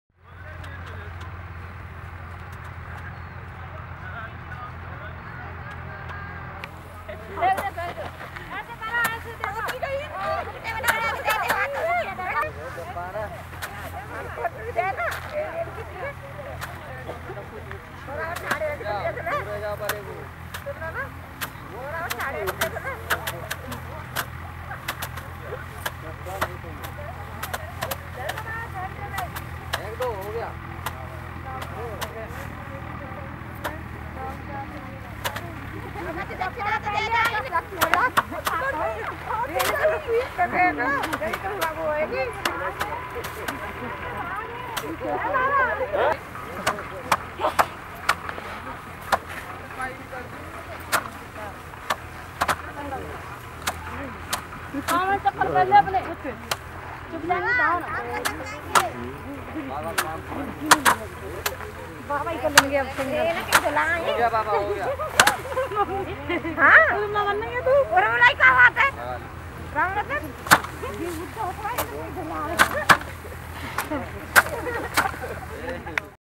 Threshing rice with locals
Local vrajavasis invited us to take part in their rice threshing.